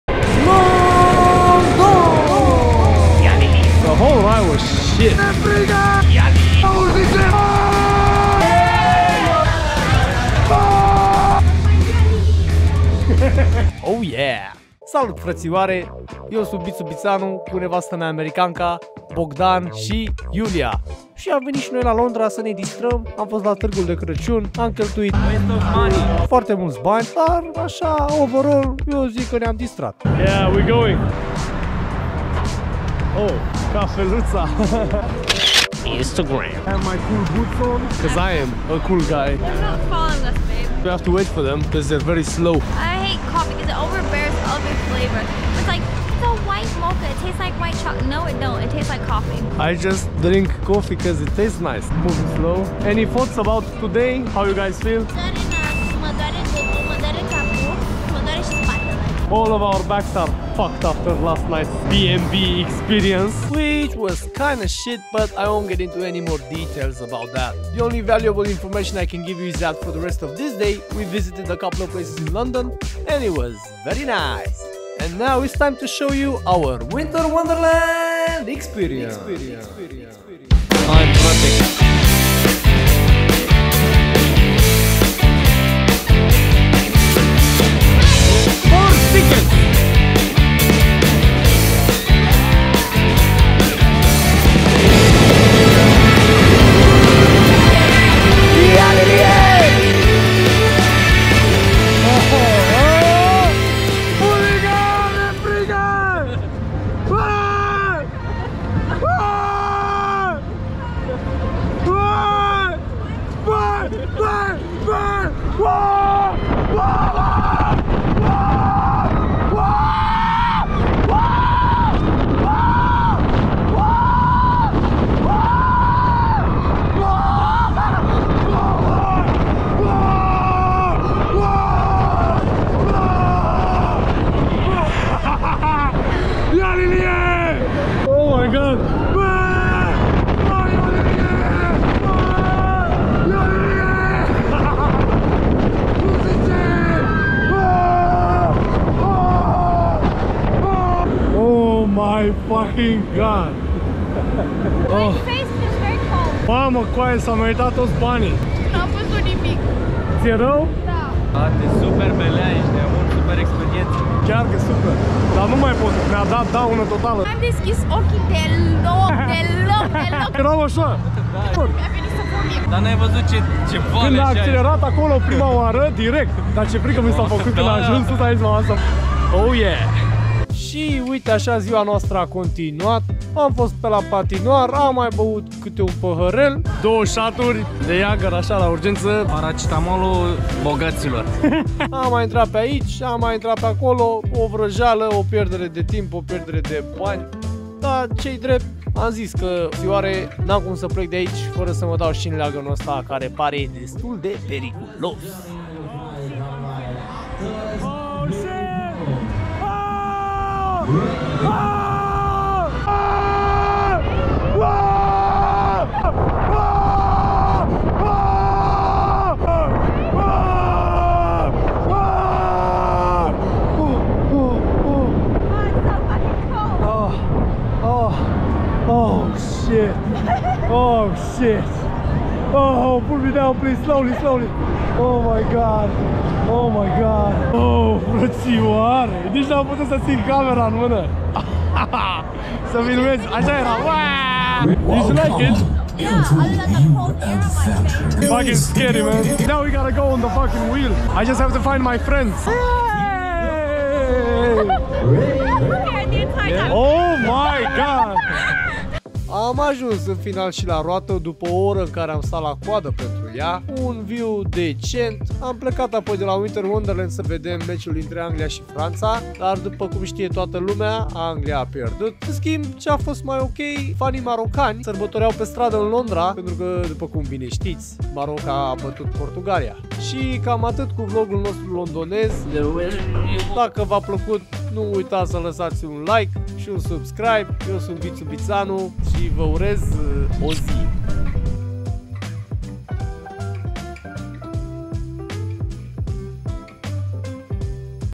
London's Winter Wonderland vlog w my friends [eng subs]
La winter wonderland cu americanca si fratii mei, ne am distrat a fost frumos n am ce lucruri rele sa zic!

Am folosit gopro 11 exclusiv. In carusel am avut horizon lock si se vede cam aiurea cand ne da peste cap, dar aia e.
Prima oara cand fac vlog, prima oara cand ies cu aceasta camera.